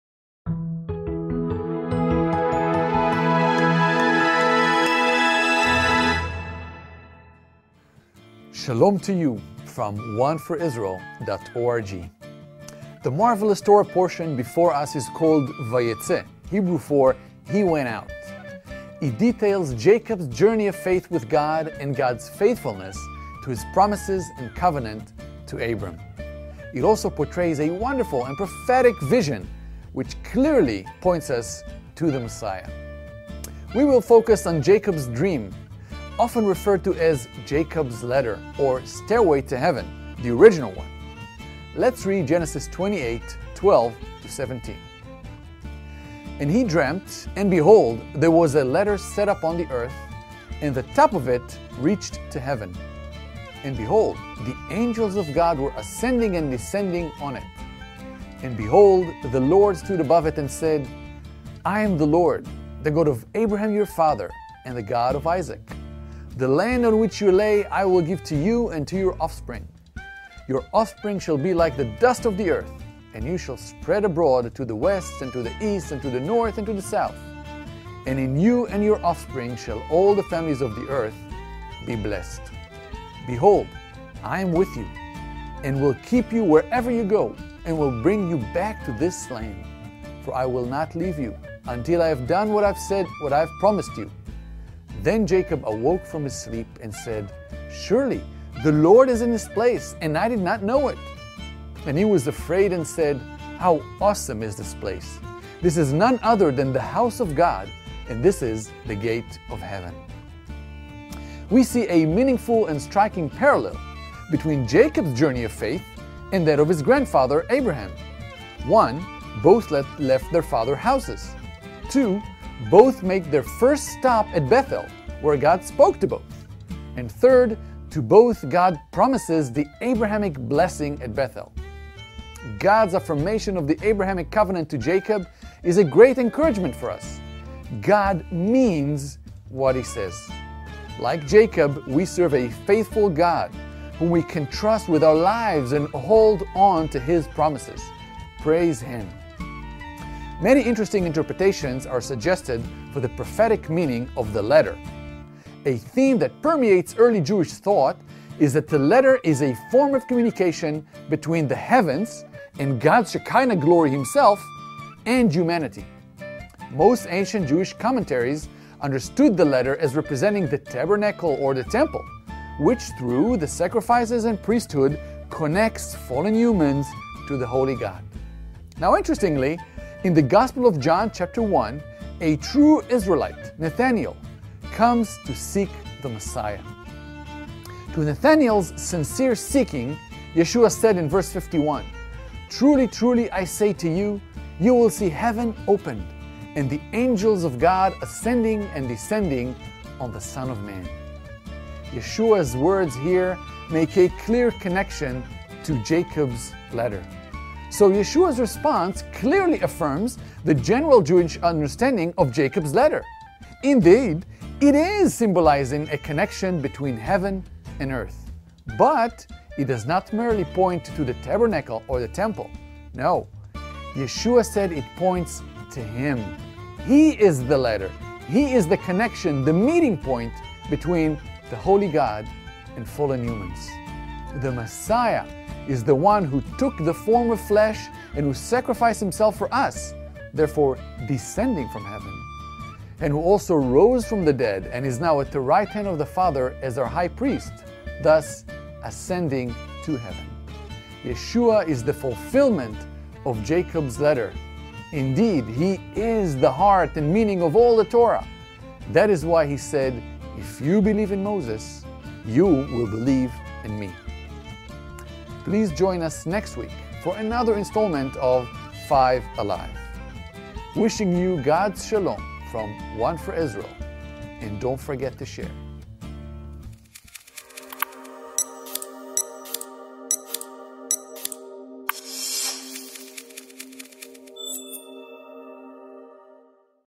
[토라포션 7주] 봐예쩨 (그리고 그가 나갔다) - 야곱의 사다리는 누구입니까?
토라 토라 (모세오경): 창세기 28:10-32:3
하프타라 (예언서): 호세아 11:7-12:12

더 많은 이들에게 복음의 기쁨 소식이 전파될수 있도록 여러분의 많은 공유 부탁드립니다.

한국사무소 전화: 서울) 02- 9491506